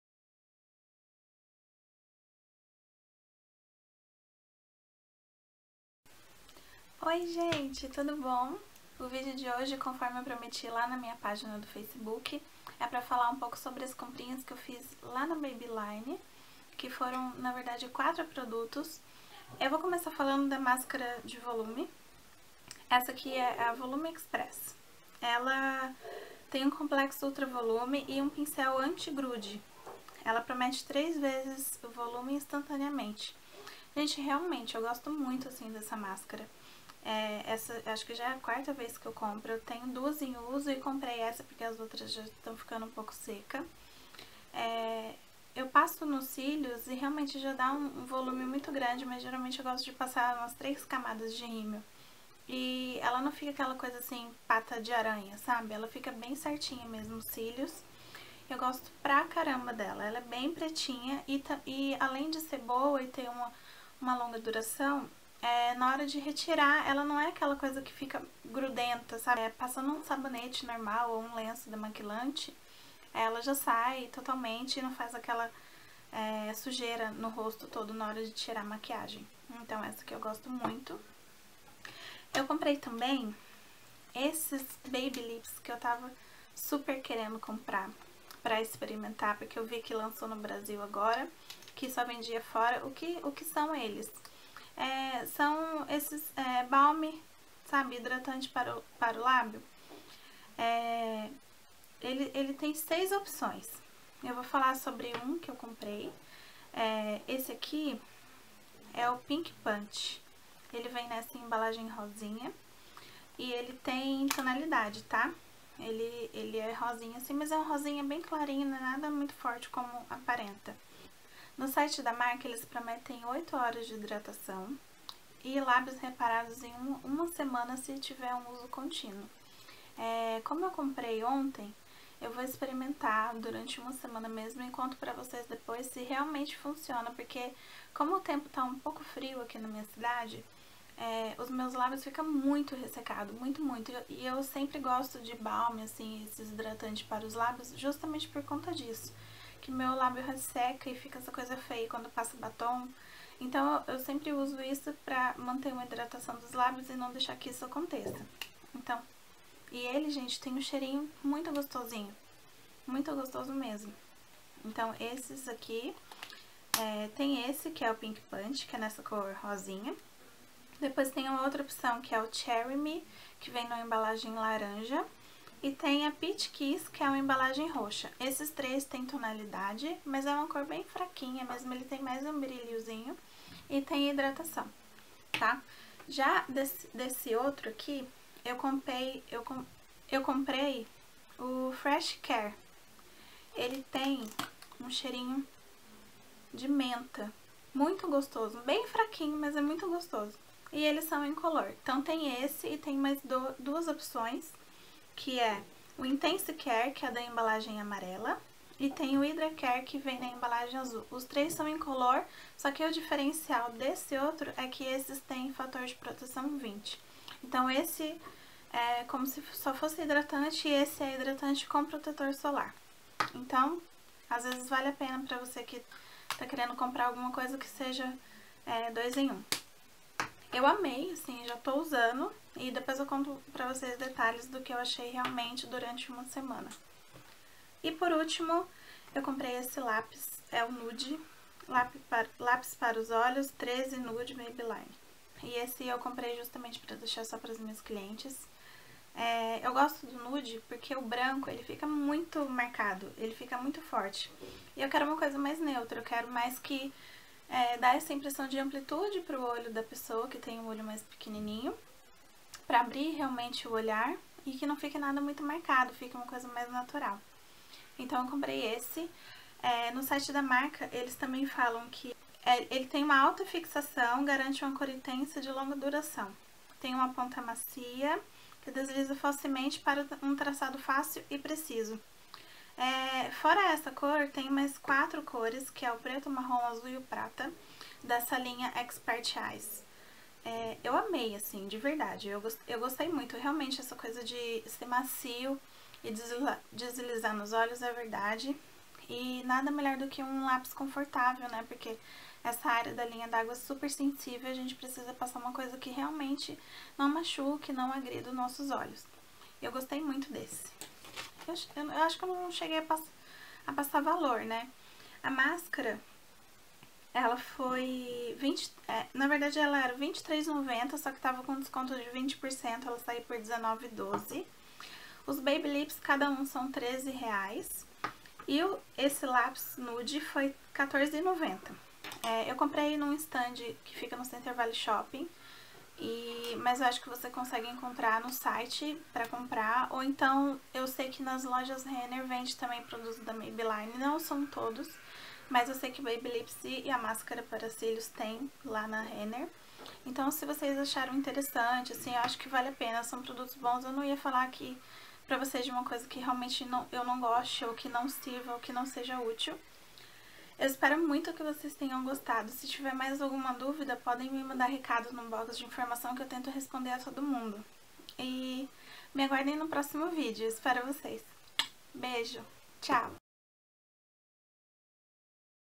Comprinhas Maybelline
Descrição dos produtos:

Máscara para cílios Volum Express Preta
Lápis delineador para os olhos Expert Eyes cor: 13 NUDE
Baby Lips Fresh Care (verde) com FPS 20
Baby Lips Pink Punch (rosa)